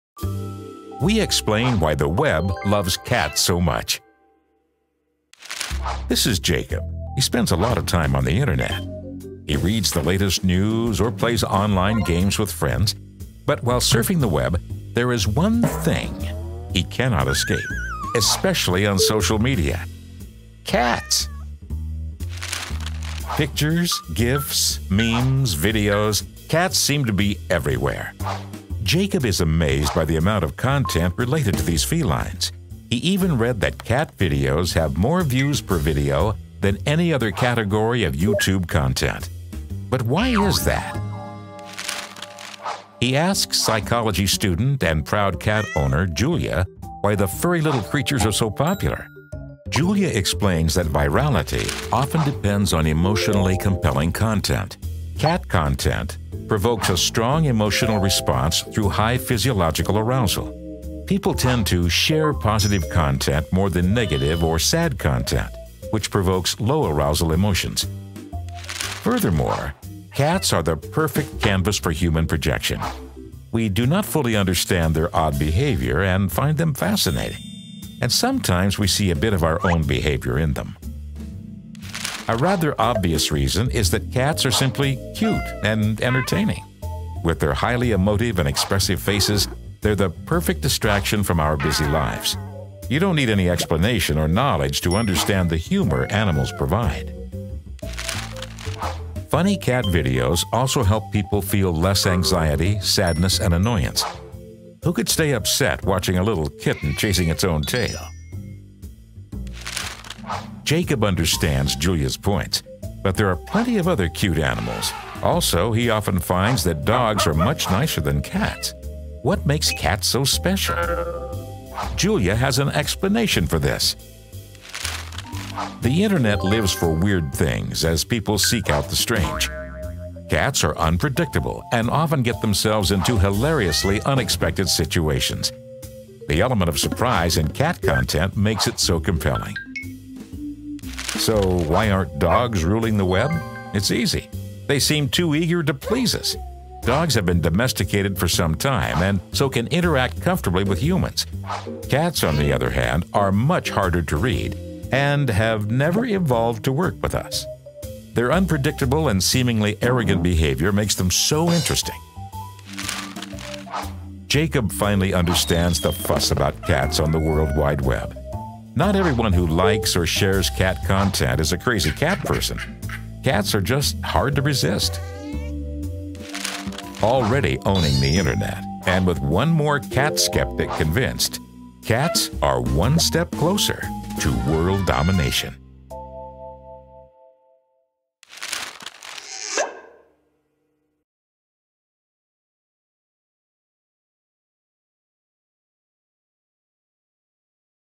Cat Content- Why the Web loves cats so much
Why does the Web love cats? And what’s Cat Content? You probaby have noticed that one animal is especially big on the Internet. Meow! Cats really took over the World Wide Web. But why are there so many cat videos, images, memes and gifs cluttering up the Social Web? What makes content-related to these felines so shareable? Watch our video to get the answers!